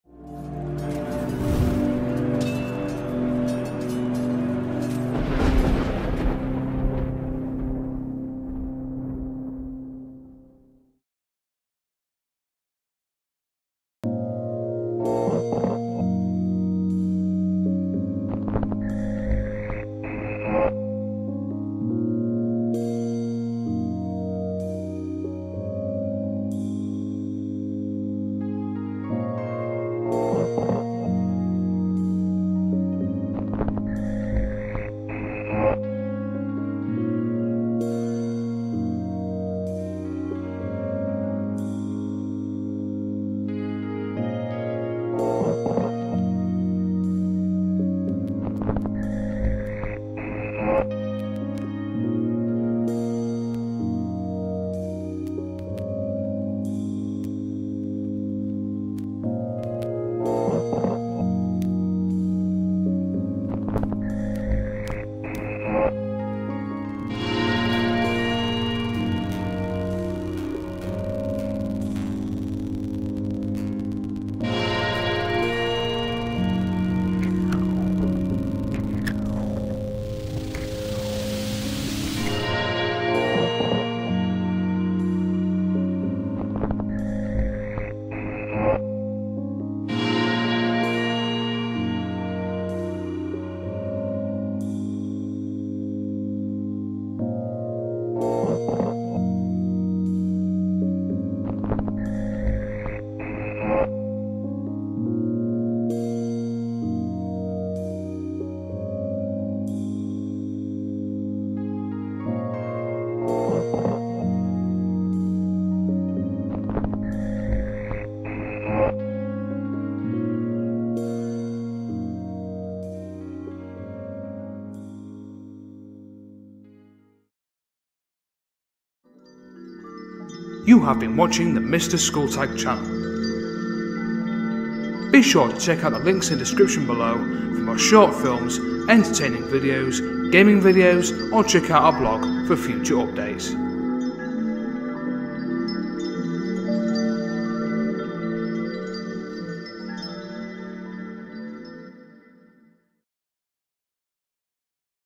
A DEAD SEASON
This is an expression, a feeling I have represented in a short video. After many years of being ruined by numberous factors I have cut myself off from a dead season.